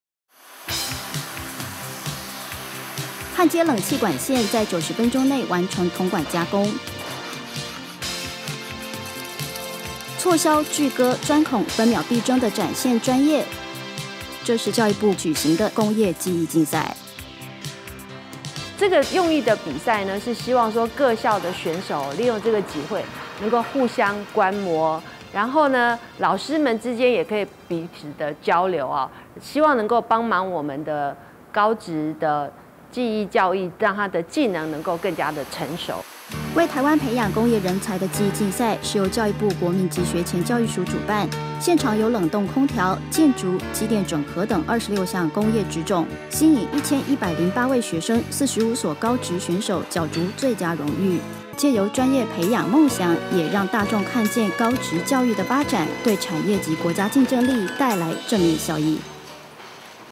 【看見教育在進步】106年全國工業類技藝競賽， 45所高職選手角逐各職種【金手獎】的最佳榮譽
106年全國工業類技藝競賽， 45所高職選手角逐各職種【金手獎】的最佳榮譽。